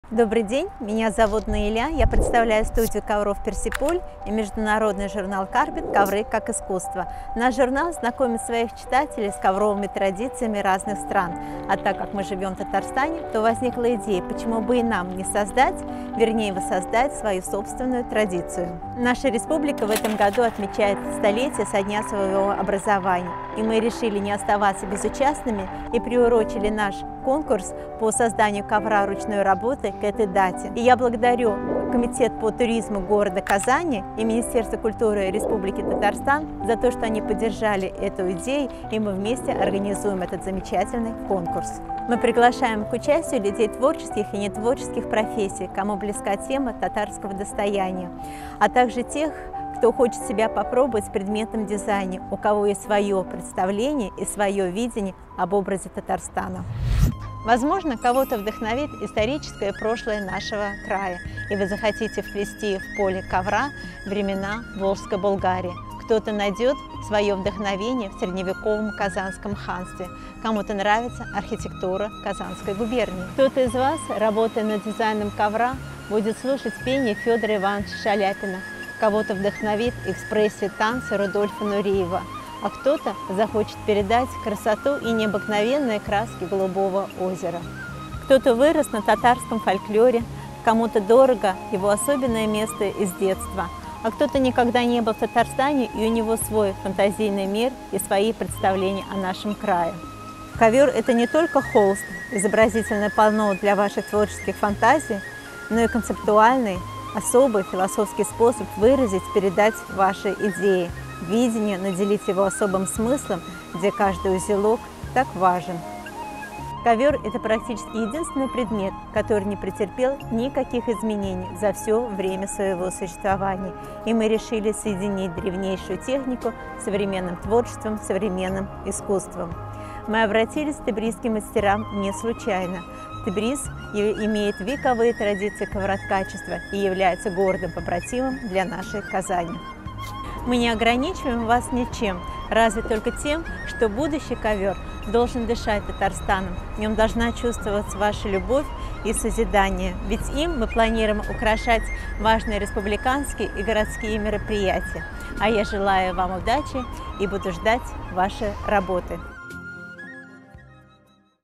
Конкурс дизайна ковра «Дух Татарстана»
Примите участие во всероссийском конкурсе дизайна ковра «Дух Татарстана»! ⠀

Студия ковров Persepol совместно с МКУ «Комитет по развитию туризма г.Казани», при информационной поддержке Министерства культуры РТ и международного журнала “CARPET: Ковры как искусство» объявляют конкурс на лучший дизайн ковра, отражающего культурно-исторические корни и традиции Татарстана.
Организаторы конкурса приглашают к участию людей творческих профессий вне зависимости от места проживания. Принимаются оформленные оригинальные дизайны ковра на тему «Дух Татарстана», это может быть как уклон в традиционное татарское творчество, затрагивающее исторические корни, так и современная интерпретация татарского наследия.
Победившая дизайнерская концепция будет реализована на ковровой мануфактуре иранского Тебриза, города-побратима Казани, при содействии Студии ковров PERSEPOL.
❗️Работы принимаются до 1 декабря 2020 г.
Победителей ждут специальные призы, культурно-развлекательный уик-енд в Казани, а также публикация в журнале “CARPET: Ковры как Искусство».
✅Готовый Ковер будет презентован в Казани, предполагается его использование на важных республиканских мероприятиях, а также возможна разработка сувенирного туристического продукта на основе победивших дизайнерских концепциях.
✅Форма представления материалов: принимаются файлы в векторном формате на , в ручном исполнении в цвете на миллиметровой бумаге формата А3-на почтовый адрес
420015 г.Казань, ул.Гоголя, 10
ООО «Персеполь»
Каждый участник может прислать до двух проектов.